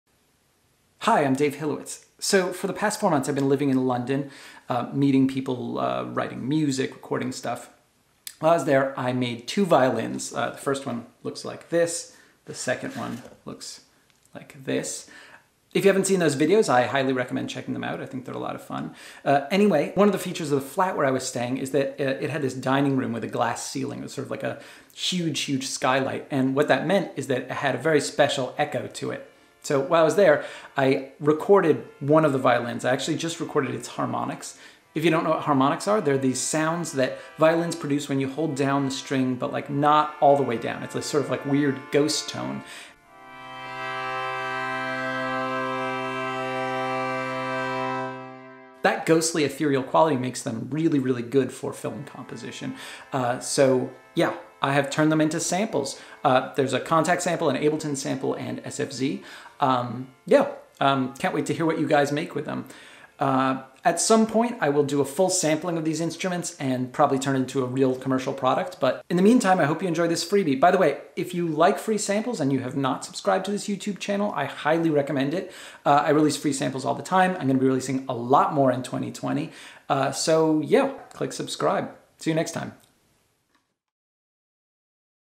Free Sample: Box Violin Harmonics (Kontakt, Ableton Live, SFZ)
For the last four months, I've been living in London — meeting people, recording stuff, writing music. While I was there, I built two violins in the flat where I was living. If you haven't seen those videos, I highly recommend checking them out (I think they're a lot of fun.) Before leaving the UK, I sampled the harmonics on one of the two violins, and now I’m releasing them as a free sample. The samples are available for Kontakt, Ableton, and SFZ.

I release free samples all the time, and this is the best way to find out about them.